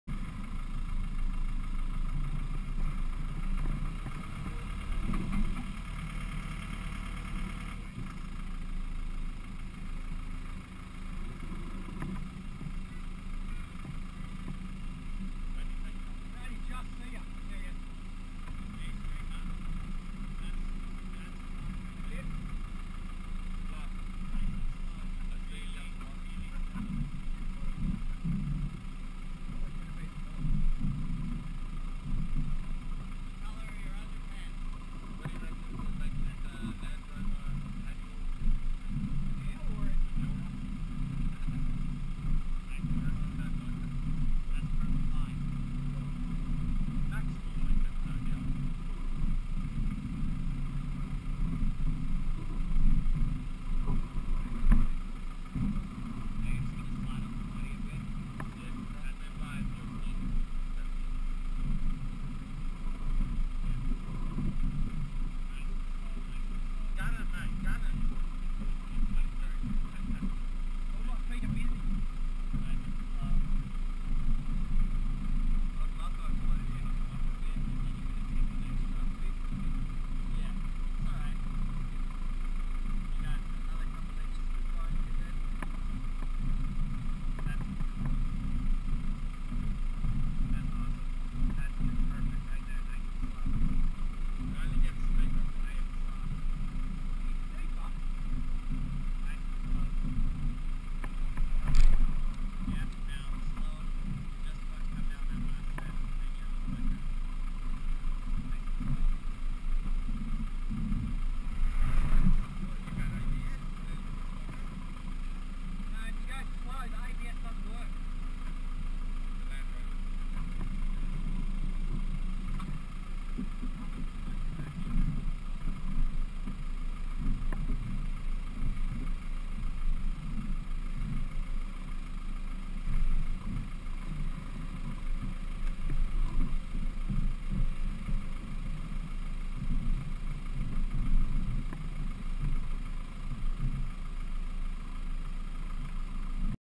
2016-05: Defender 110 Gees Arm South - Stage 11 descent - Toolbox Hill
Stock Land Rover Defender 110 on Gees Arm South Trail at Wheeney Creek, descending Toolbox Hill
I could not see my spotter until he moved over around the 17 second mark
GoPro Wide HD, roof mount view
Wheeney Creek, NSW, Australia